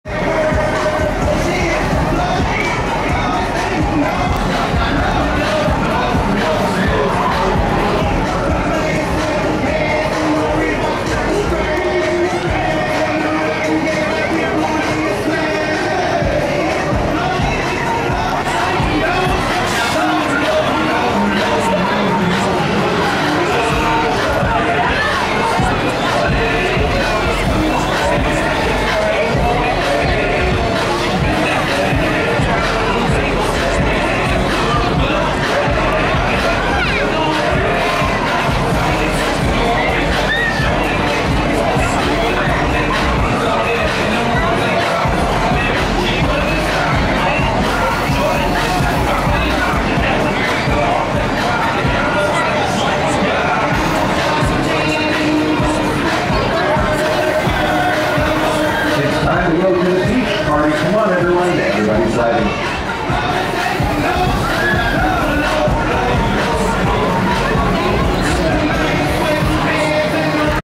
🗽🏙️New York New York Funhouse at the 2022 Wisconsin State Fair
New York New York Funhouse at the 2022 Wisconsin State Fair in West Allis, Wisconsin, USA. This New York New York Funhouse is Manufactured by GosettoIt transports on two trailers.